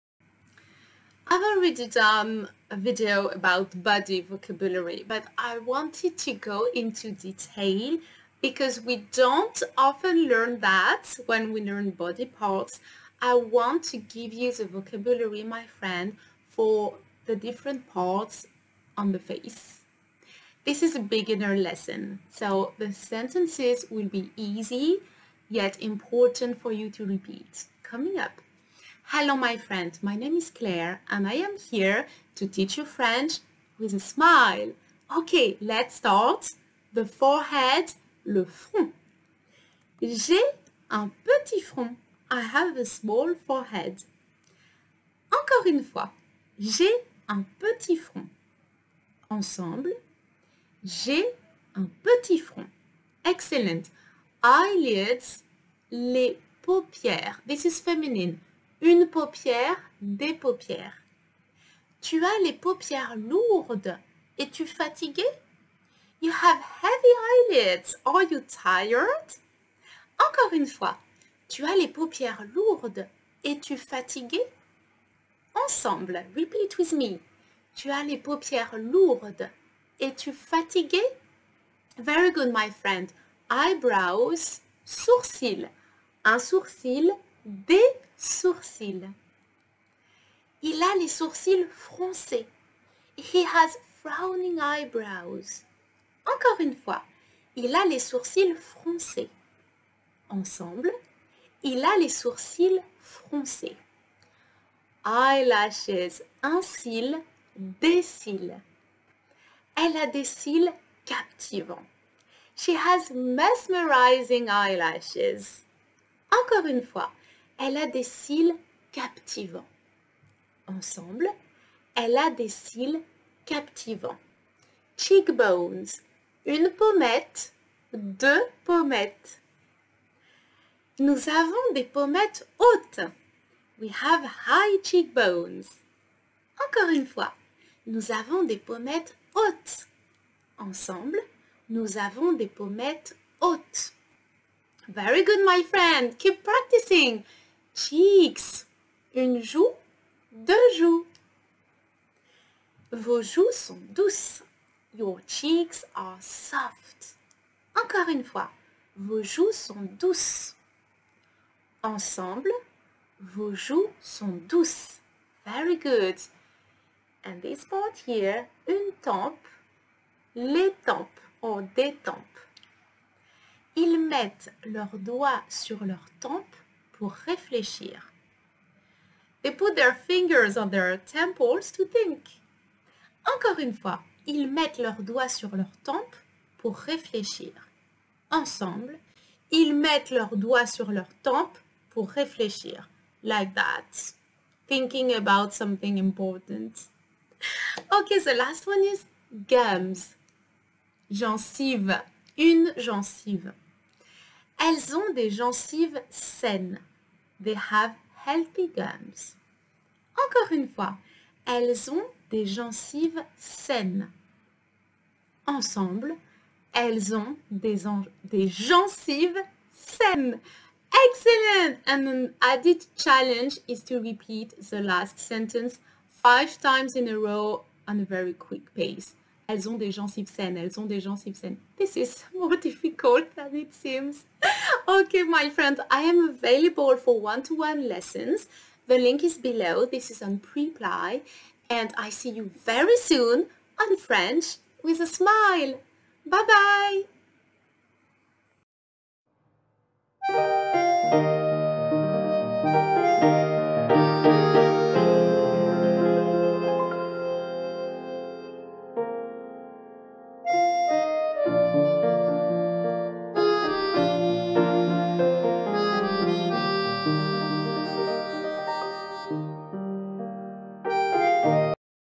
Beginner #20 Face parts vocabulary and sentences to pronounce and repeat in French or le visage
Learn how to name each part of your face in detail with French words and simple phrases my friend.

Book a one-to-one lesson with me on Preply (search for Claire Samuel)

J'ai un petit front.
I have a small forehead.

Tu as les paupières lourdes, es-tu fatigué ?
You have heavy eyelids, are you tired?

Il a les sourcils froncés.
He has frowning eyebrows.

Elle a des cils captivants.
She has mesmerizing eyelashes.

Nous avons des pommettes hautes.
We have high cheekbones.

Vos joues sont douces.
Your cheeks are soft.

Ils mettent leurs doigts sur leur tempes pour réfléchir.
They put their fingers on their temples to think.

Elles ont des gencives saines.
They have healthy gums.

Please remember that all opinions are my own.